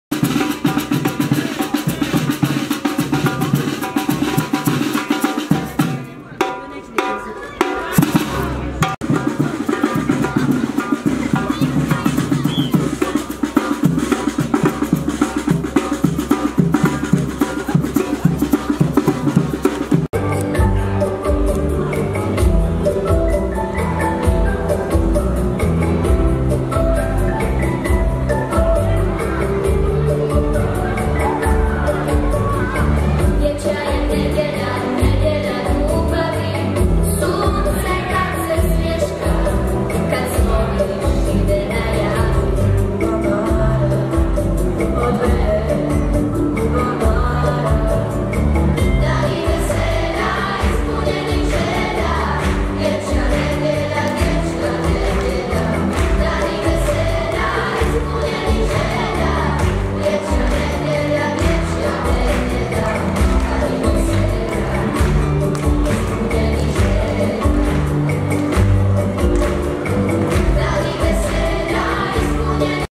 Počela dječija nedjelja pod motom 'Sretan ćeš biti, svijete, kad usrećiš dijete'
SARAJEVO, 2. oktobra (FENA) - Dječija nedjelja u Kantonu Sarajevo počela je danas svečanim programom u Domu mladih pod motom ''Sretan ćeš biti, svijete, kad usrećiš dijete''.